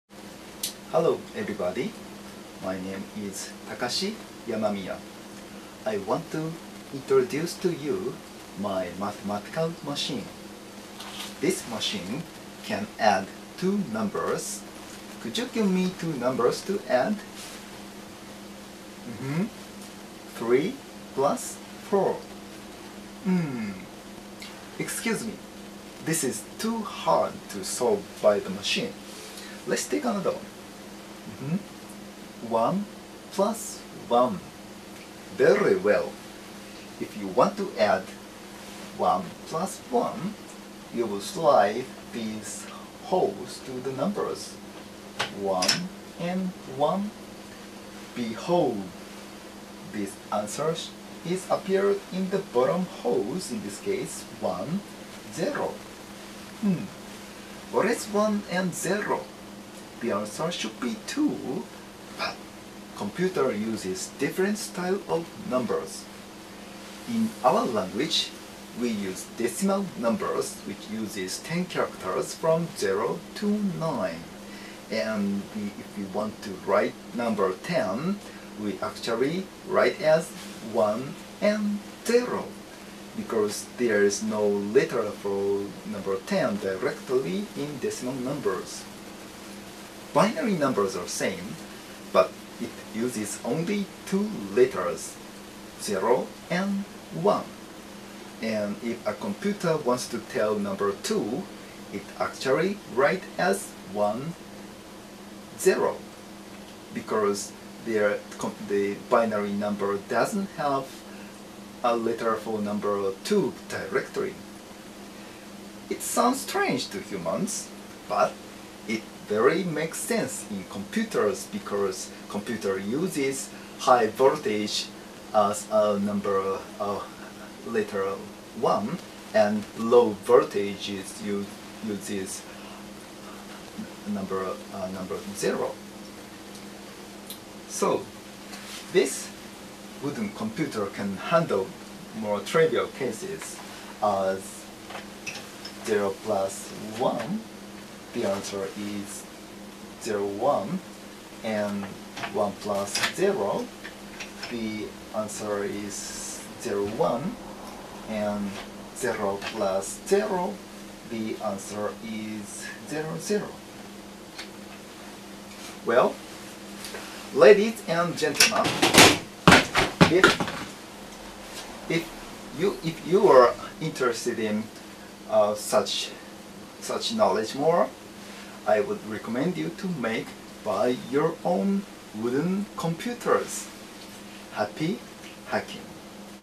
The Wooden Half Adder (test version)
Explanation of the wooden half adder and binary numbers